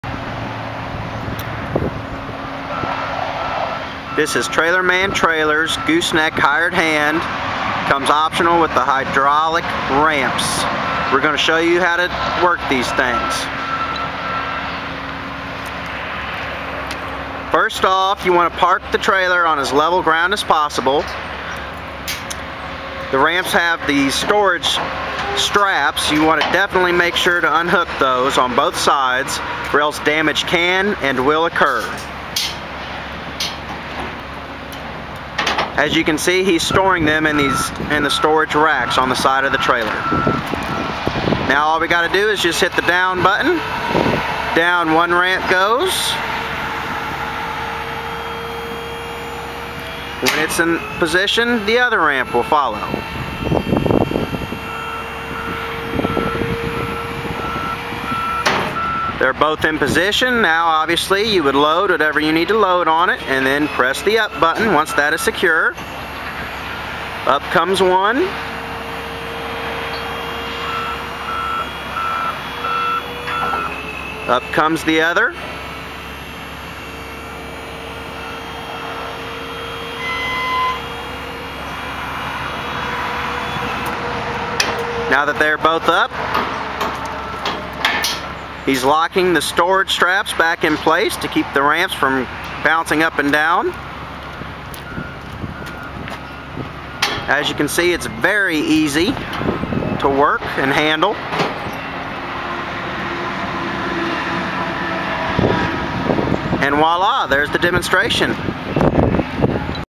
Trailerman® Hydraulic Ramps
These ramps come as an option on our Gooseneck Hired Hand model trailer.